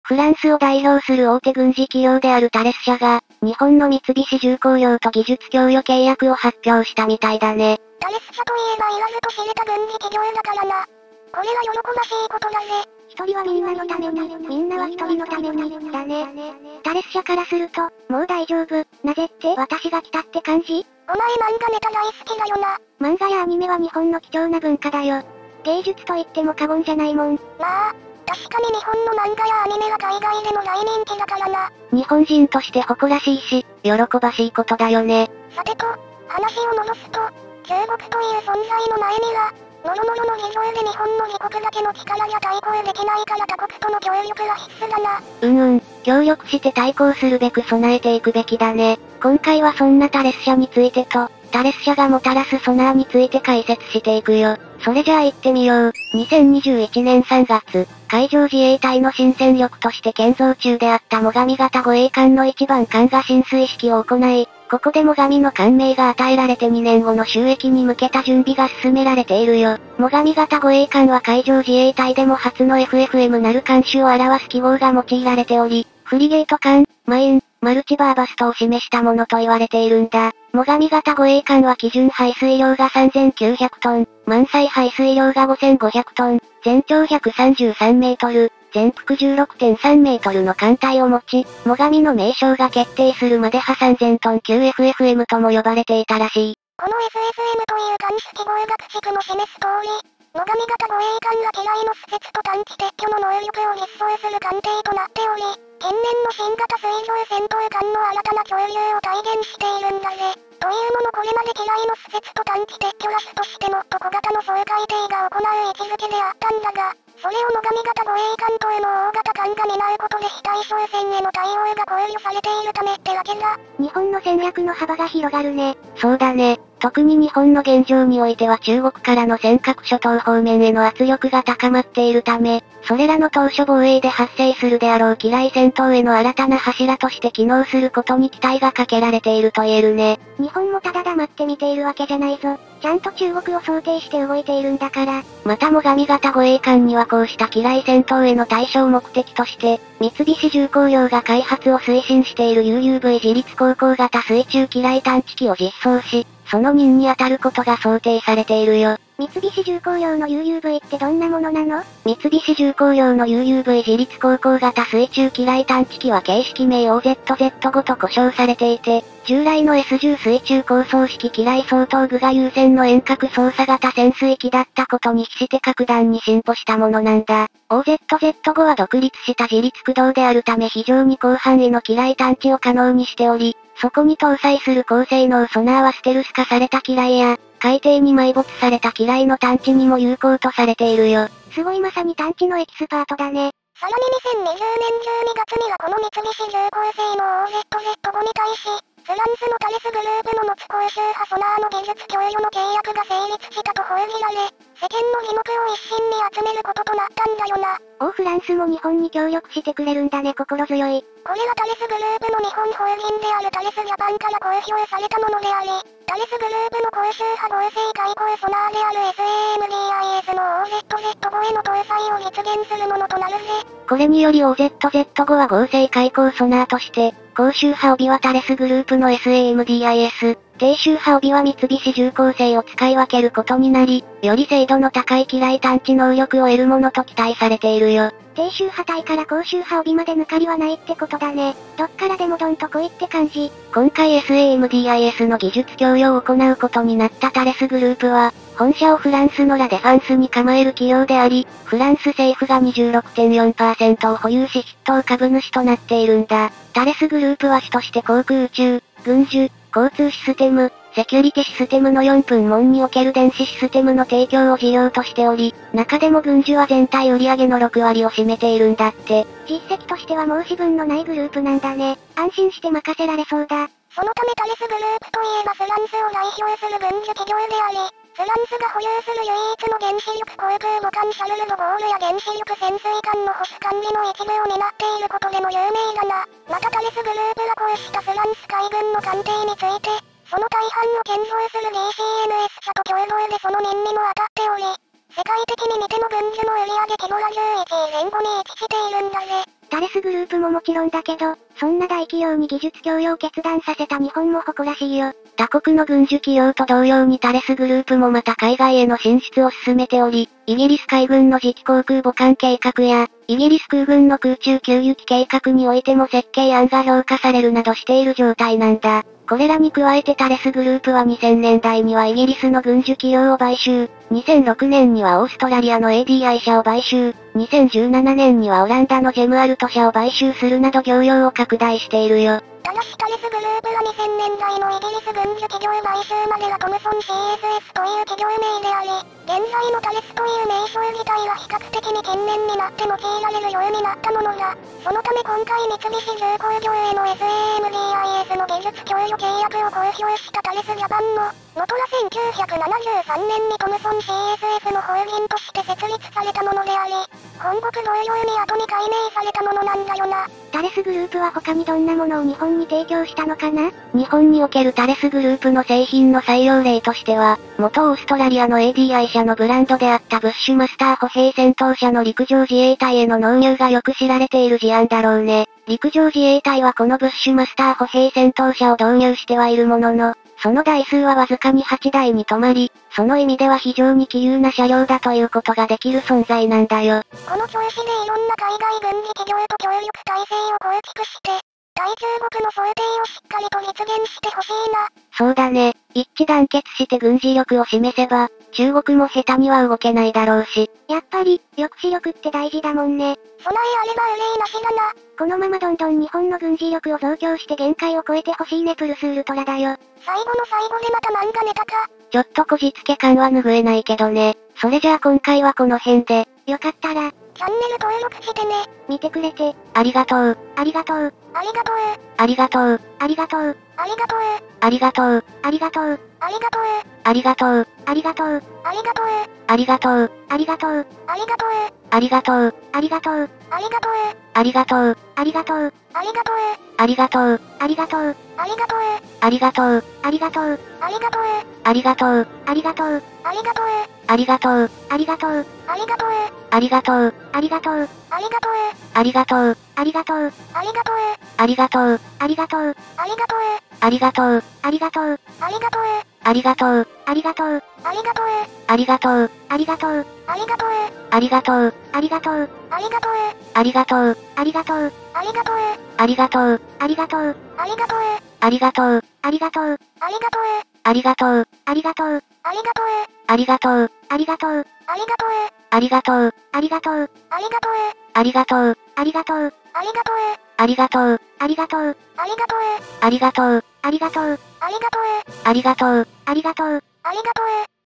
仏タレス社と三菱重工業が技術提携！新型UUA・「OZZ-5」周波合成開口ソナーを強化した性能は？【ゆっくり解説】
引用・転載「出典：海上自衛隊ホームページ」
引用・転載「出典：航空自衛隊ホームページ」
引用・転載「出典：陸上自衛隊ホームページ」